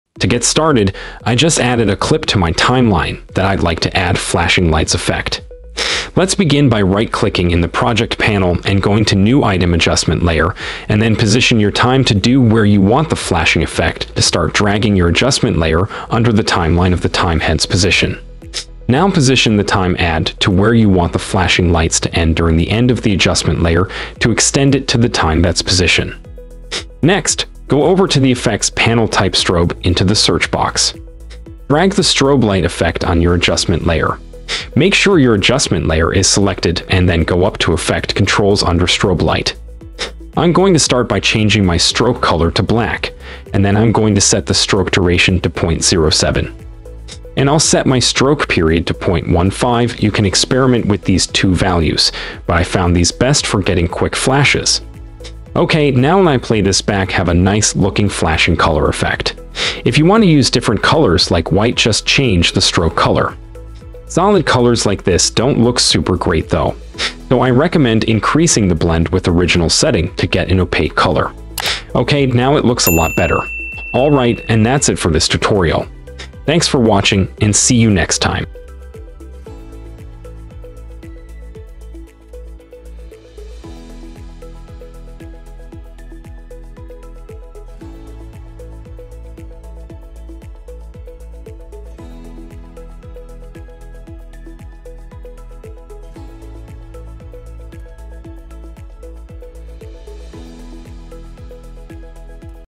How to Flashing Lights Effect Premiere Pro 2024 | Premiere Pro Tutorials
Learn the art of creating captivating Flashing Lights Effects in Premiere Pro 2024 with this step-by-step tutorial. Make your videos stand out and leave your audience in awe with this easy-to-follow guide.
How to Flashing Lights Effect Premiere Pro 2024.